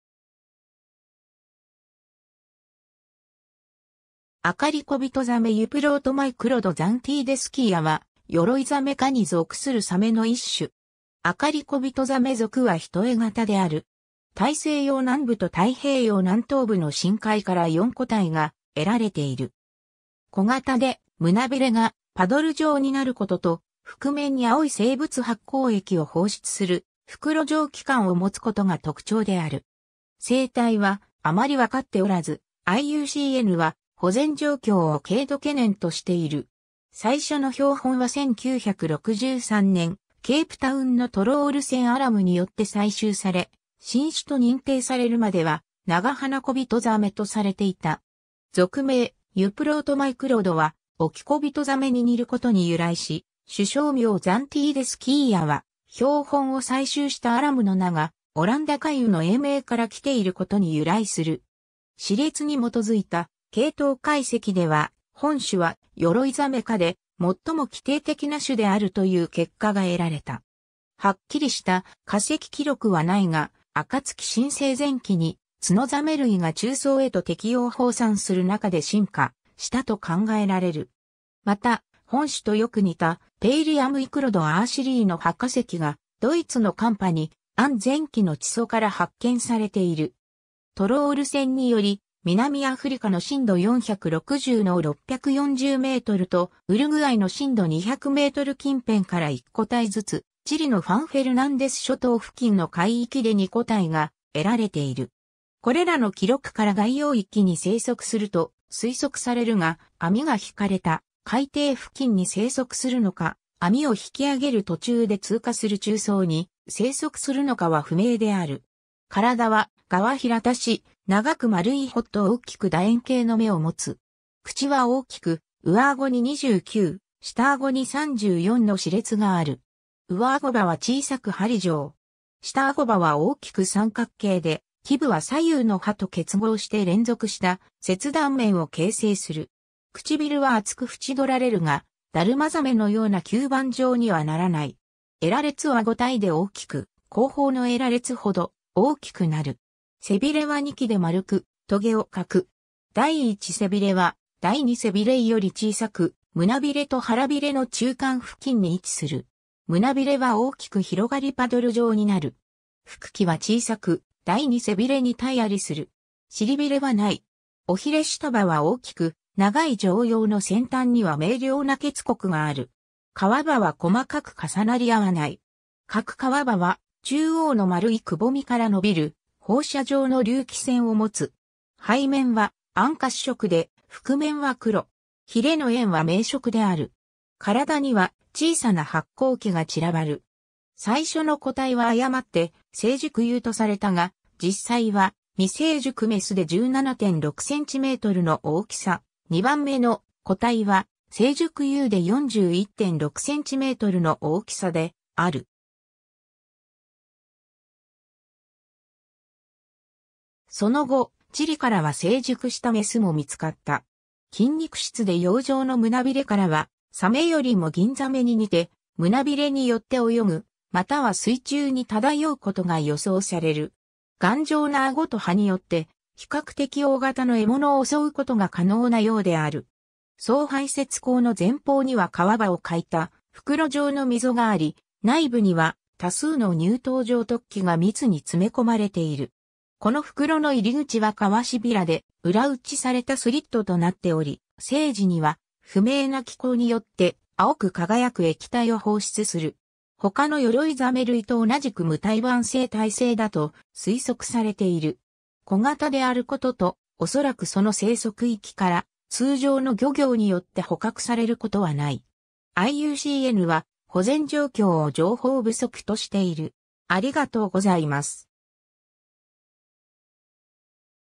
アカリコビトザメ
アカリコビトザメ Euprotomicroides zantedeschia はヨロイザメ科に属するサメの一種。
アカリコビトザメ属は単型である。
大西洋南部と太平洋南東部の深海から4個体が得られている。
小型で、胸鰭がパドル状になることと、腹面に青い生物発光液を放出する袋状器官を持つことが特徴である。
生態はあまり分かっておらず、IUCNは保全状況を軽度懸念としている。
 最初の標本は1963年、ケープタウンのトロール船 Arum によって採集され、新種と認定されるまではナガハナコビトザメとされていた。
属名 Euprotomicroides はオキコビトザメ (Euprotomicrus bispinatus) に似ることに由来し、種小名 zantedeschia は、標本を採集した Arum の名がオランダカイウ (Zantedeschia aethiopica) の英名から来ていることに由来する。
 歯列に基づいた系統解析では、本種はヨロイザメ科で最も基底的な種であるという結果が得られた。
はっきりした化石記録はないが、暁新世前期（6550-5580万年前）にツノザメ類が中層へと適応放散する中で進化したと考えられる。
また、本種とよく似たPalaeomicroides ursulae の歯化石がドイツのカンパニアン前期の地層から発見されている。
 トロール船により、南アフリカの深度460-640mとウルグアイの深度200m近辺から1個体ずつ、チリのファン・フェルナンデス諸島付近の海域で2個体が得られている。
これらの記録から外洋域に生息すると推測されるが、網が曳かれた海底付近に生息するのか、網を引き上げる途中で通過する中層に生息するのかは不明である。
 体は側扁し、長く丸い吻と大きく楕円形の眼を持つ。
口は大きく、上顎に29、下顎に34の歯列がある。
上顎歯は小さく針状。
下顎歯は大きく三角形で、基部は左右の歯と結合して連続した切断面を形成する。
唇は厚く縁取られるが、ダルマザメのような吸盤状にはならない。
鰓裂は5対で大きく、後方の鰓裂ほど大きくなる。
 背鰭は2基で丸く、棘を欠く。
第一背鰭は第二背鰭より小さく、胸鰭と腹鰭の中間付近に位置する。
胸鰭は大きく広がりパドル状になる。
腹鰭は小さく、第二背鰭に対在する。
臀鰭はない。
尾鰭下葉は大きく、長い上葉の先端には明瞭な欠刻がある。
皮歯は細かく重なり合わない。
各皮歯は、中央の丸い窪みから伸びる放射状の隆起線を持つ。
背面は暗褐色で腹面は黒。
鰭の縁は明色である。
体には小さな発光器が散らばる。
最初の個体は誤って成熟雄とされたが、実際は未成熟雌で17.6cmの大きさ、2番目の個体は成熟雄で41.6cmの大きさである。
その後、チリからは成熟した雌も見つかった。
 筋肉質で葉状の胸鰭からは、サメよりもギンザメに似て、胸鰭によって泳ぐ、または水中に漂うことが予想される。
頑丈な顎と歯によって、比較的大型の獲物を襲うことが可能なようである。
総排泄孔の前方には皮歯を欠いた袋状の溝があり、内部には多数の乳頭状突起が密に詰め込まれている。
この袋の入口は皮褶で裏打ちされたスリットとなっており、生時には不明な機構によって青く輝く液体を放出する。
他のヨロイザメ類と同じく無胎盤性胎生だと推測されている。
 小型であることと、おそらくその生息域から、通常の漁業によって捕獲されることはない。
IUCNは保全状況を情報不足としている。